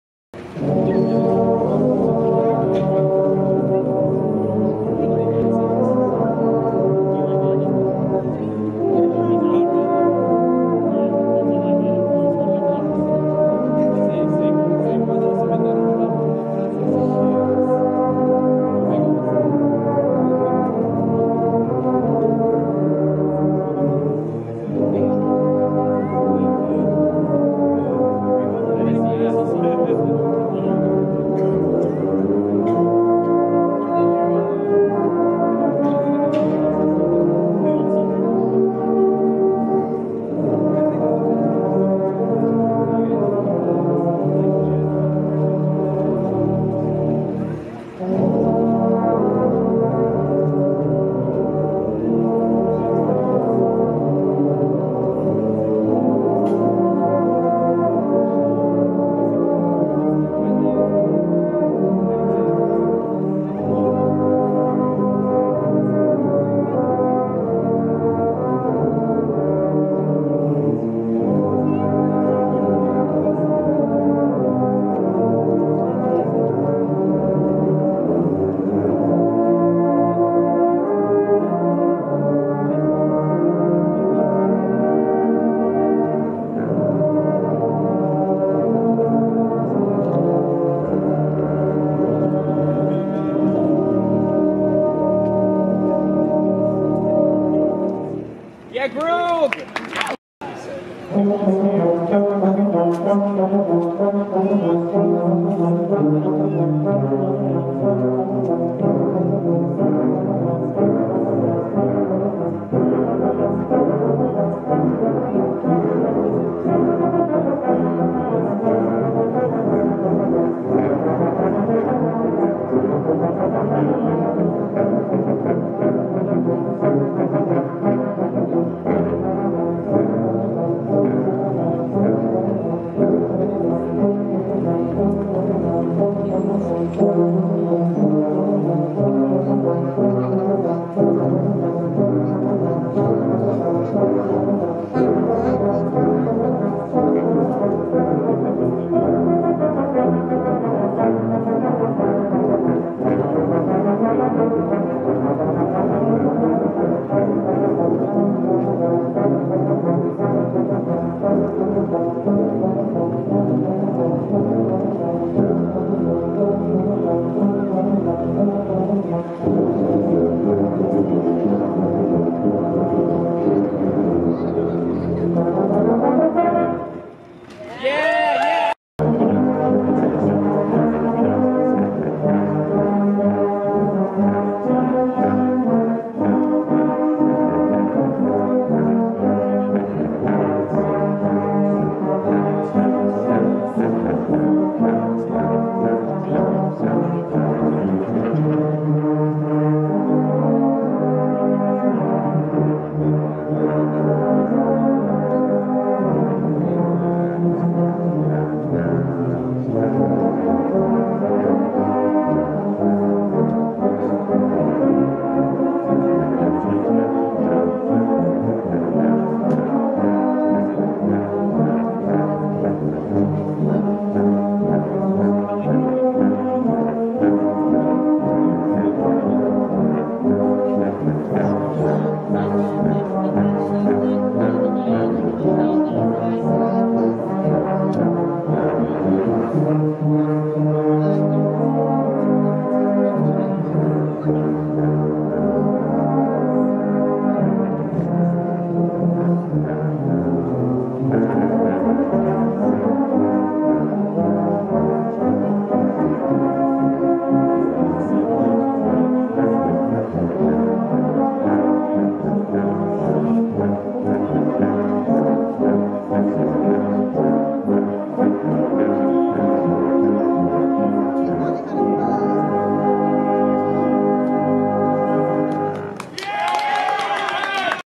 Tuba Christmas 2019 in Pittsburgh's Schenley Plaza
12/7/19 Tubas perform Christmas songs in Pittsburgh's Schenley Plaza as part of Tuba Christmas!